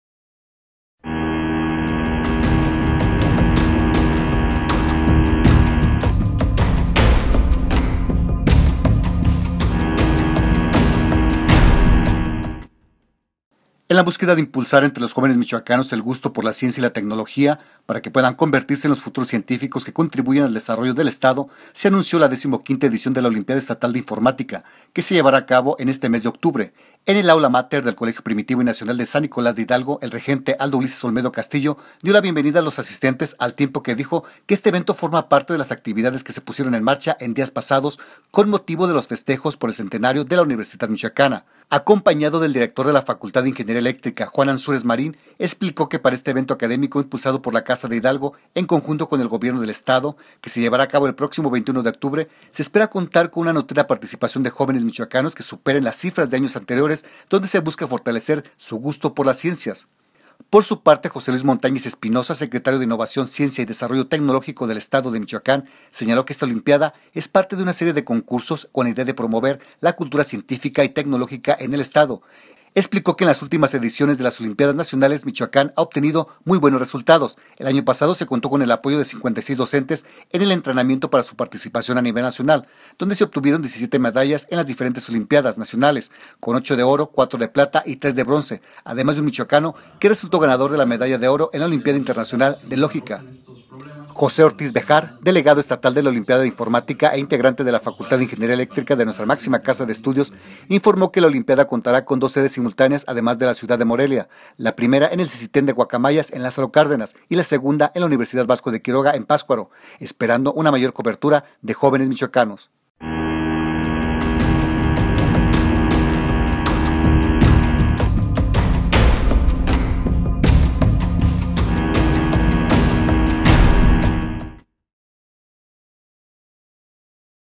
XV OLIMPIADA DE INFORMATICA
En la búsqueda de impulsar entre los jóvenes michoacanos el gusto por la ciencia y la tecnología, para que puedan convertirse en los futuros científicos que contribuyan al desarrollo del estado, se anunció la XV edición de la Olimpiada Estatal de Informática que se llevará a cabo en este mes de octubre.

En el aula mater del Colegio Primitivo y Nacional de San Nicolás de Hidalgo, el regente Aldo Ulises Olmedo castillo, dio la bienvenida a los asistentes, al tiempo que dijo que este evento forma parte de las actividades que se pusieron en marcha en días pasados, con motivo de los festejos por el Centenario de la Universidad Michoacana,

Acompañado del director de la Facultad de Ingeniería Eléctrica Juan Anzúrez Marín, explicó que para este evento académico impulsado por la Casa de Hidalgo, en conjunto con el Gobierno del Estado, que se llevará a cabo el próximo 21 de octubre, se espera contar con una nutrida participación de jóvenes michoacanos que superen las cifras de años anteriores, donde se busca fortalecer su gusto por las ciencias.